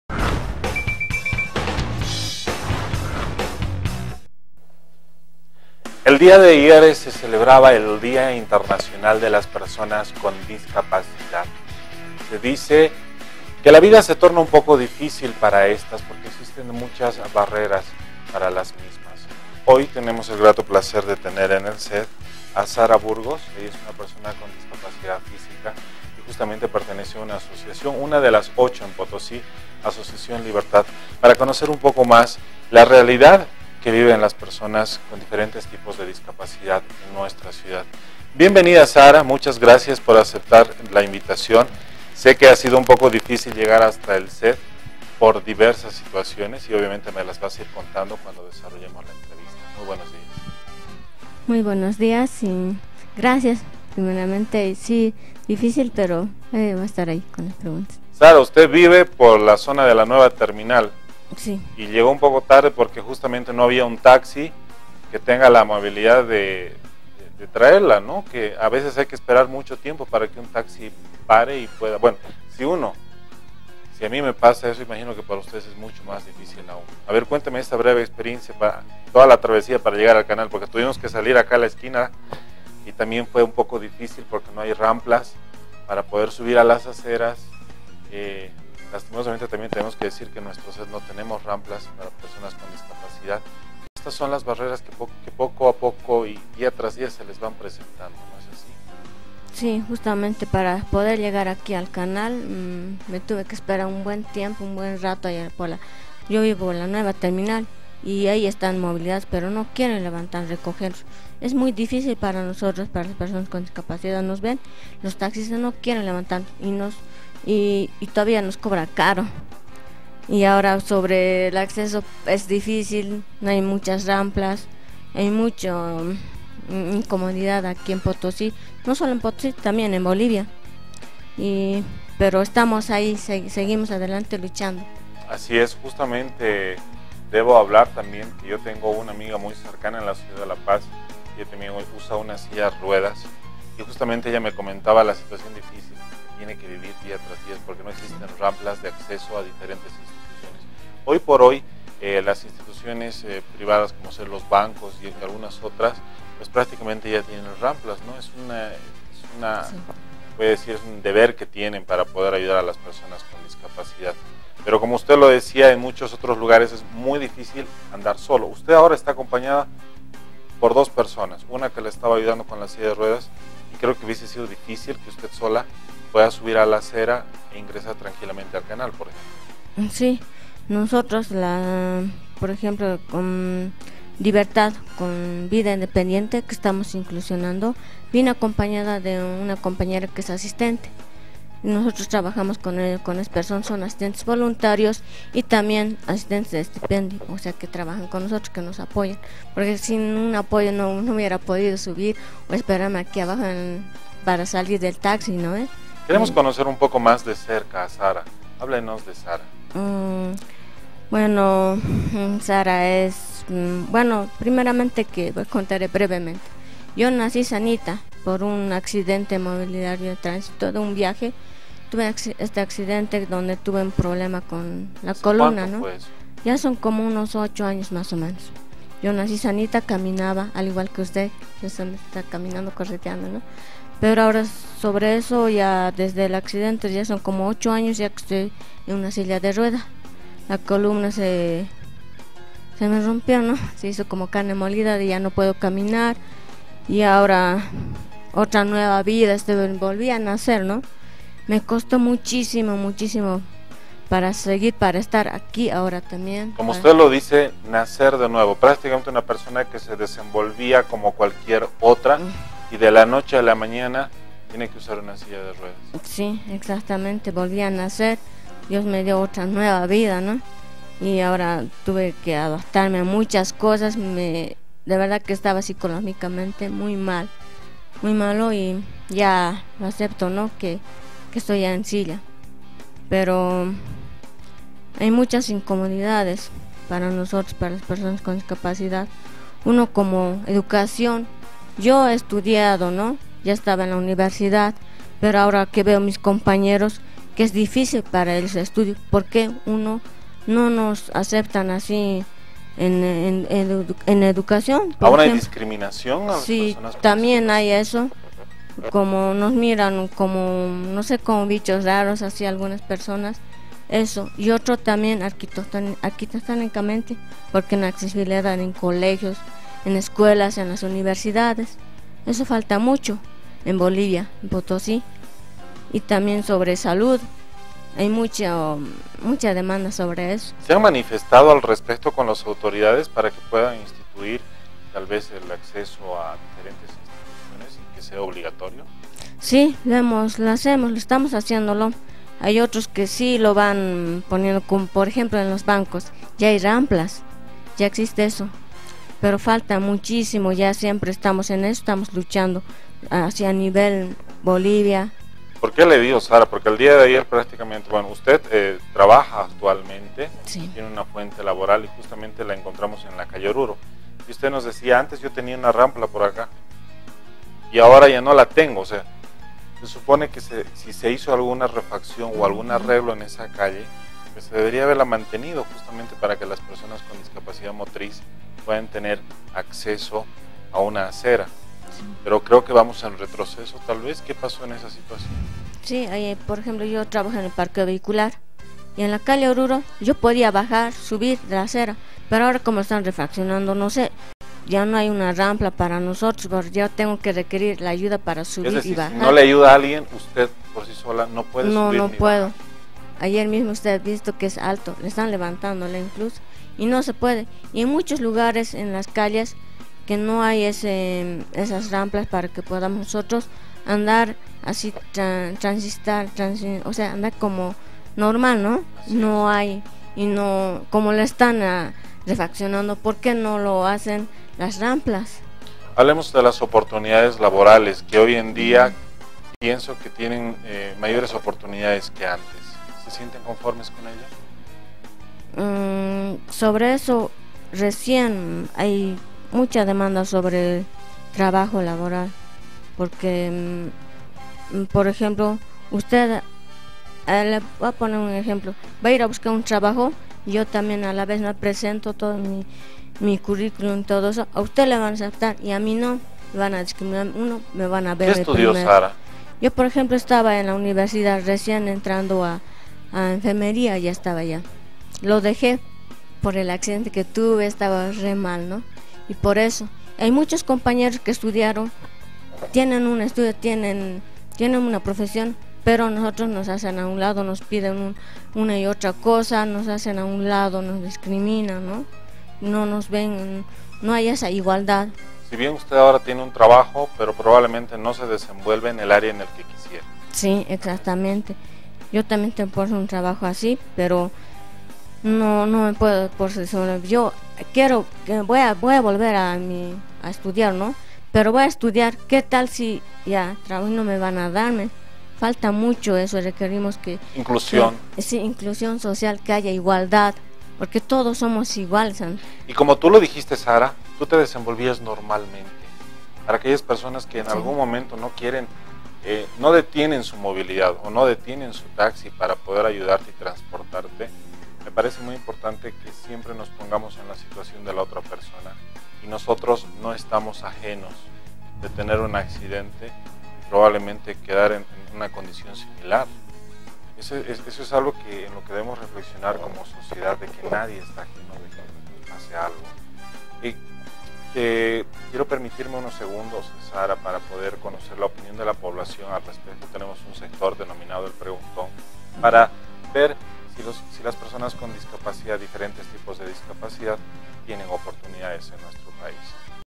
TEMA DEL DÍA: PERSONAS CON DISCAPACIDAD
Cada 3 de diciembre se conmemora mundialmente el Día Internacional de las Personas con Discapacidad.